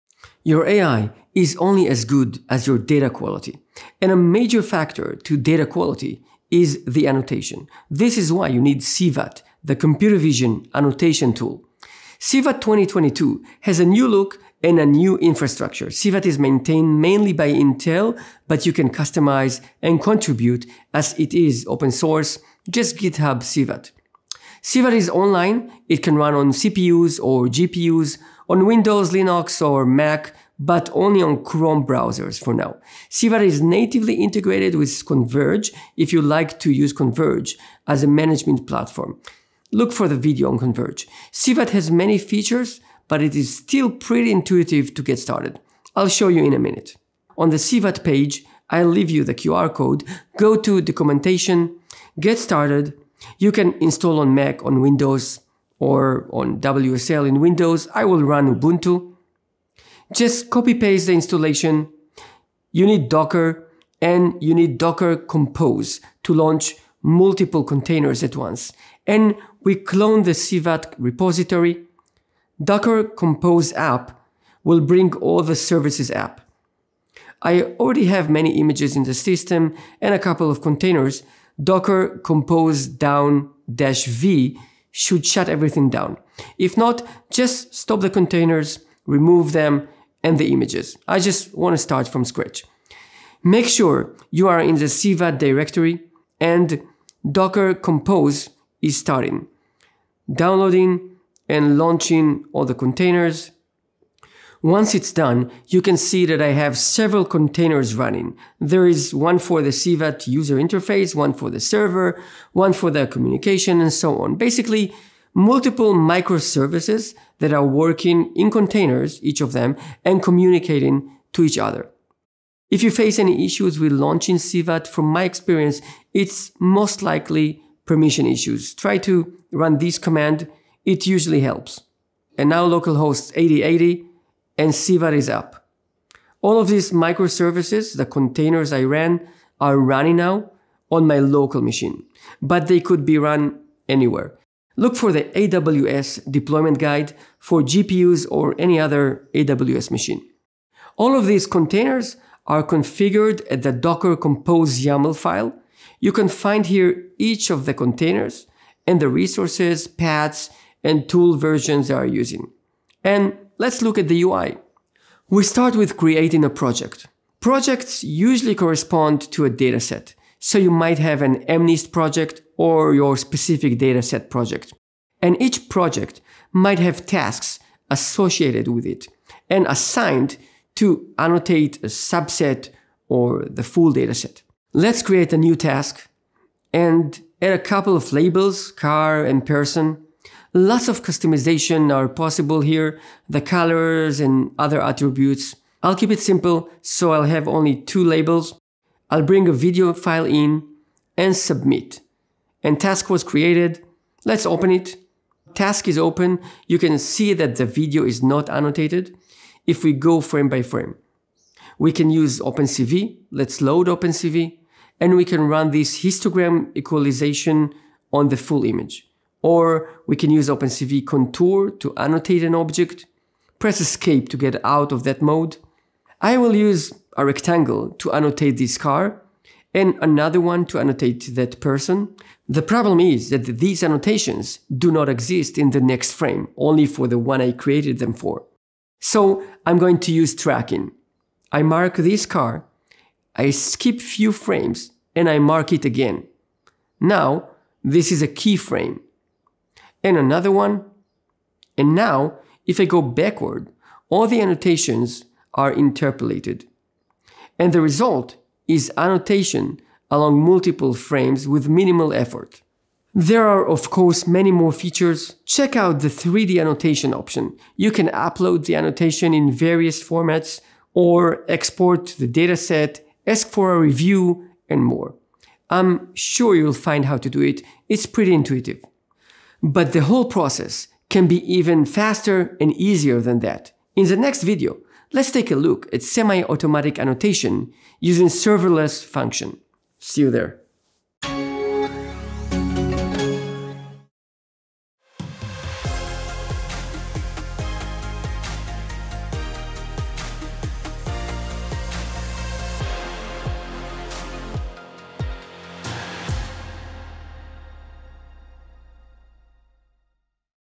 Introduction to the Computer Vision Annotation Tool (CVAT) | Intel Software
EOL 4/11/2023

CVAT is a free, online, interactive video and image annotation tool for computer vision. Intuitive to get started, flexible for advanced features.

CVAT 2022 has a new look and a new infrastructure. CVAT is free, open-source, online, and can work on multiple platforms and operating systems. In this video, we'll present how to get started and the main features. CVAT supports supervised machine learning tasks pertaining to object detection, image classification, image segmentation, and 3D data annotation.